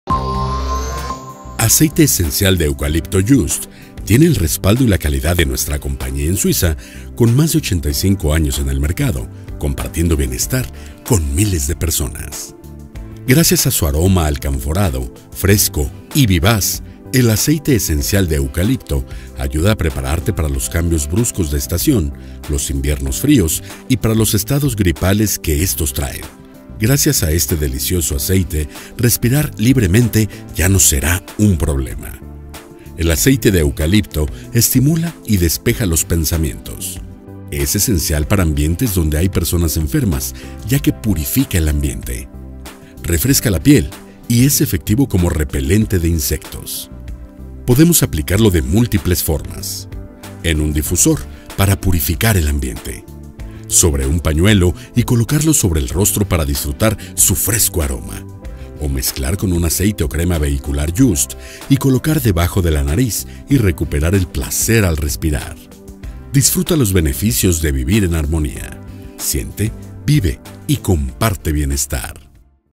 Aceite Esencial de Eucalipto Just
Aceite Esencial de Eucalipto
El Placer de Respirar Libremente

Posee un aroma inequívoco y personal. Alcanforado, fresco y vivaz, el Aceite Esencial de Eucalipto, ayuda a prepararse para los cambios bruscos de estación, los inviernos fríos y para los estados gripales que éstos traen consigo. Respirar libremente ya no será un problema.

Puedes utilizar este aceite solo o combinado con otros aceites esenciales vehiculizados en la Línea Aromablends.